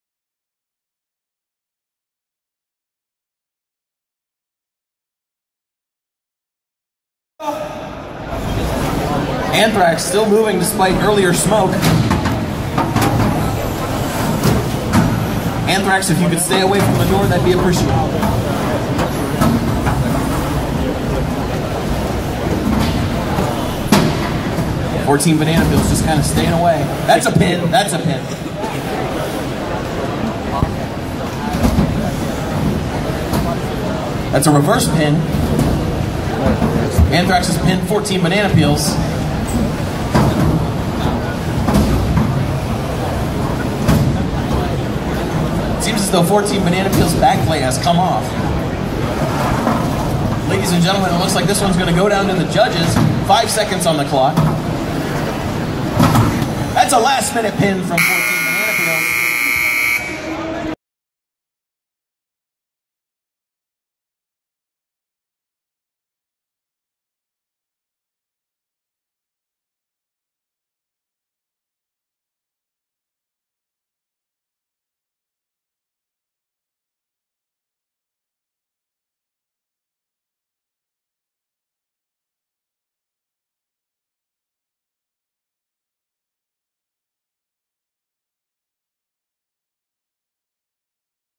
2017 Bots IQ - Cal HS - 2nd Match
California HS: 14 Banana Peels vs. Southmoreland HS: ANTHRAX